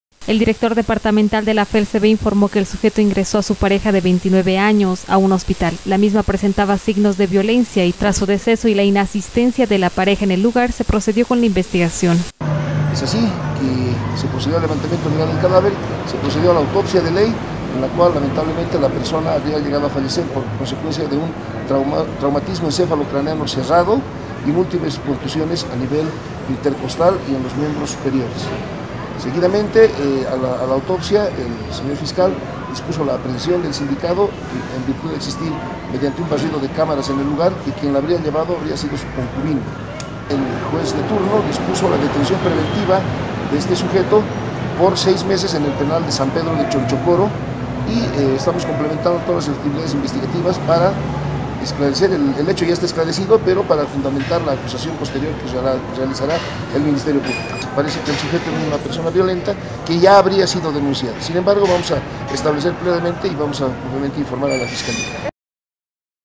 SUJETO ES APREHENDIDO TRAS PROVOCAR LA MUERTE DE SU PAREJA
Sujeto es aprehendido tras provocar el desceso de su pareja en la zona Periférica.